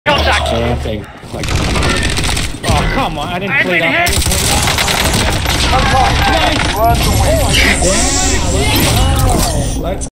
Win in Battle Royale Mini Quads
Improbable last kill after a rather anonymous game